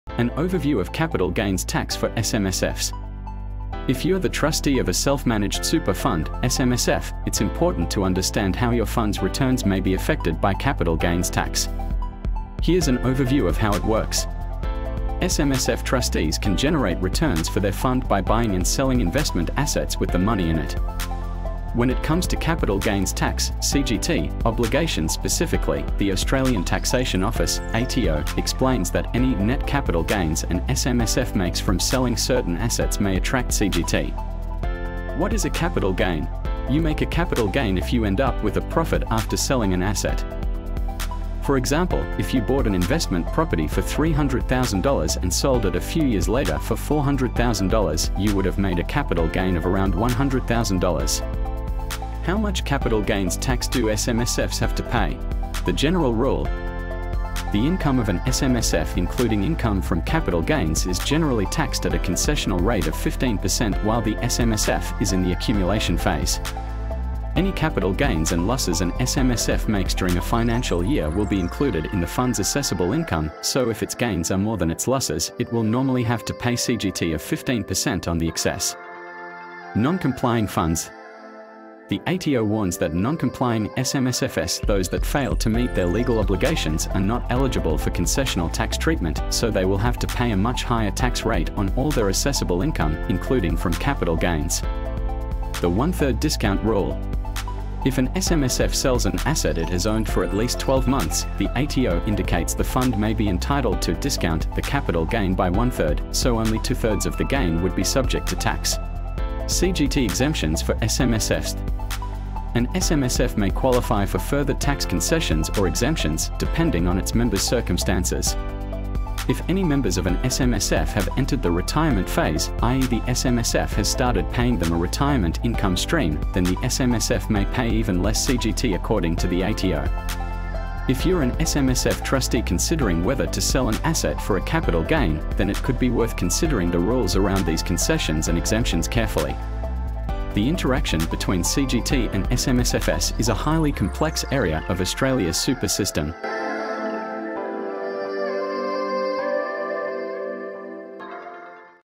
An overview of capital gains tax for SMSFs
The interplay between CGT and SMSFs is a particularly intricate aspect of Australia's financial system.

It is crucial to understand how capital gains tax may impact the returns of your fund.

Speak to one of our experts today to help ensure your funds.